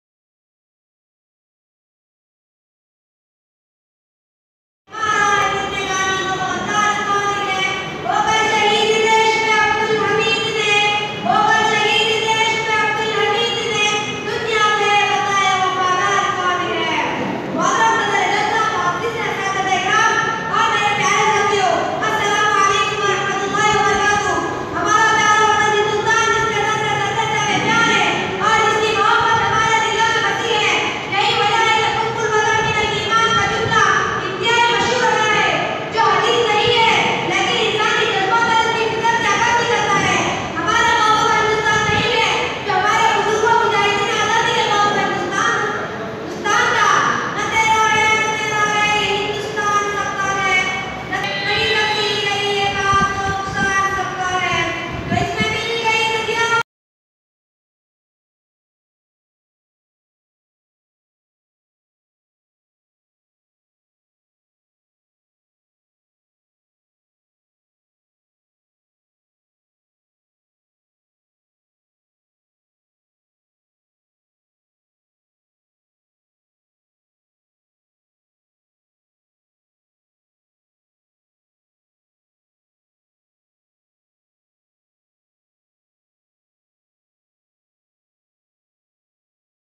Vip function hall Aurangabad me hua Jalse Aam- Ek bahetarin Takhreer
Puna ke ulema maulana nasim Chisty sahab bhi hue mavjud.
Jama masjid Aurangabad.